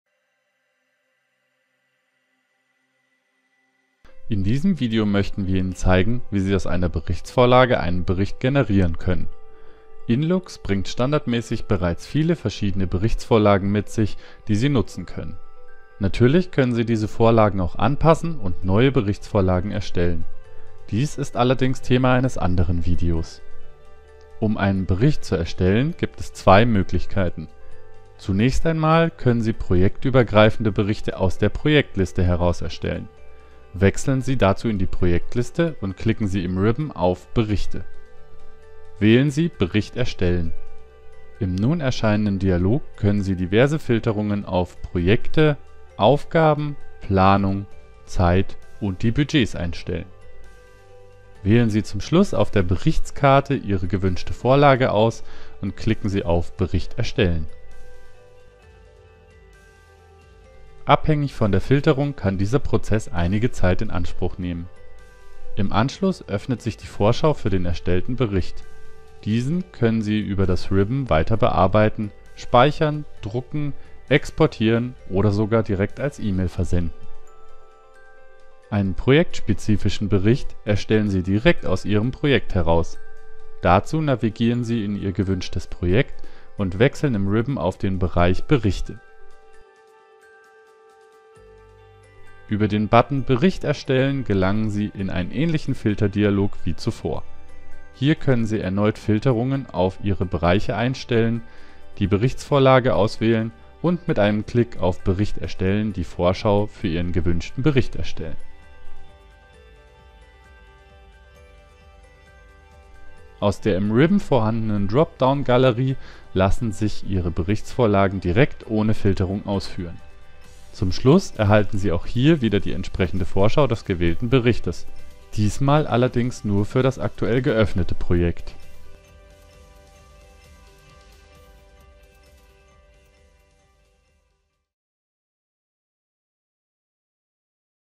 Video-Tutorial: Berichte erstellen in InLoox 10 für Outlook
So erstellen Sie Berichte in InLoox 10 für Outlook